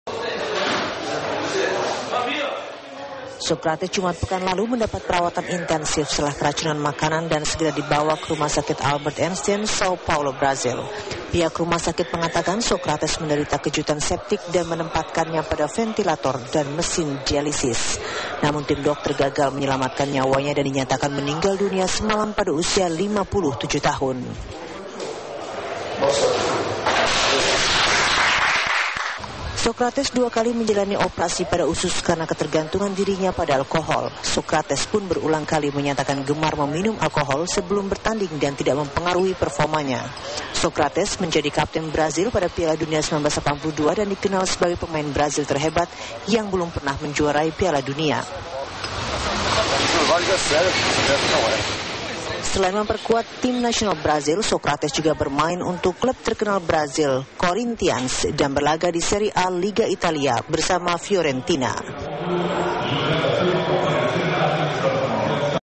VIVAnews : Legenda Sepak Bola Brasil, Socrates Tutup Usia
Brasil tengah berduka. Salah satu legenda sepak bola mereka baru saja meninggal. Socrates yang merupakan kapten tim nasional Brasil pada Piala Dunia 1982 meninggal di Sao Paulo pada Minggu pagi, 4 Desember 2011.

SidoMuncul 60 Tahun ( 11-11-51 to 11-11-11 )